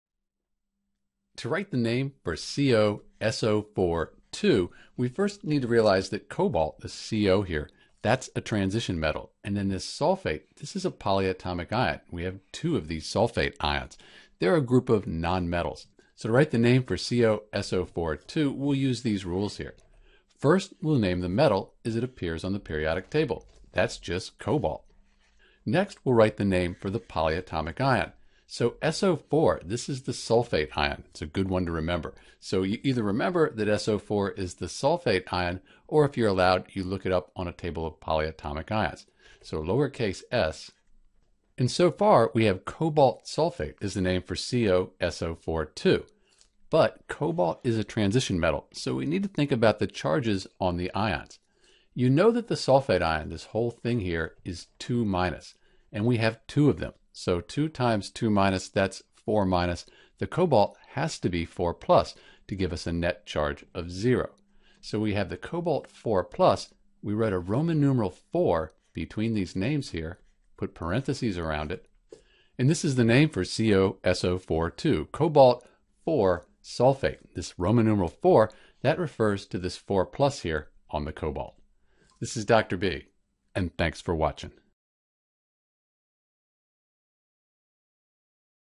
How to Write the Name for Co(SO4)2
In this video we'll write the correct name for Co(SO4)2. To write the name for Co(SO4)2 we’ll use the Periodic Table and follow some simple rules.

Because Co(SO4)2 has a transition metal we need to indicate the ionic charge using Roman numerals in parenthesis.

1) Write the name of transition metal as it appears on the Periodic Table.

2) Write the name and charge for the non-metal. If you have a polyatomic ion, use the Common Ion Table to find and write the formula and charge.

3) Use the total charge on the non-metal (or polyatomic ion) find the charge on the transition metal.

4) After the name for the metal, write its charge as a Roman Numeral in parentheses. Example: Iron (III) chloride

For ionic compounds like Cobalt (IV) sulfate, you should check to make sure the charges balance to make sure you have the right name.

For a complete tutorial on naming and formula writing for compounds, like Cobalt (IV) sulfate and more, visit: